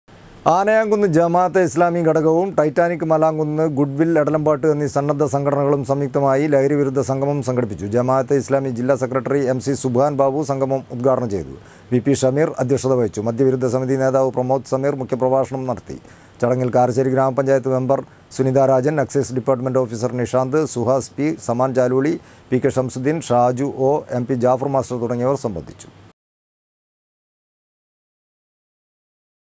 ലഹരിവിരുദ്ധ സംഗമം സംഘടിപ്പിച്ചു
ആനയാംകുന്ന് ജമാഅത്തെ ഇസ്ലാമി ഘടകവും, ടൈറ്റാനിക് മലാംകുന്ന്, ഗുഡ് വിൽ എടലമ്പാട് എന്നീ സന്നദ്ധ സംഘടനയും സംയുക്തമായി, ലഹരിവിരുദ്ധ സംഗമം സംഘടിപ്പിച്ചു. ജമാഅത്തെ ഇസ്ലാമി ജില്ലാ സെക്രട്ടറി എംസി സുബ്ഹാൻ ബാബു സംഗമം  ഉദ്ഘാടനം ചെയ്തു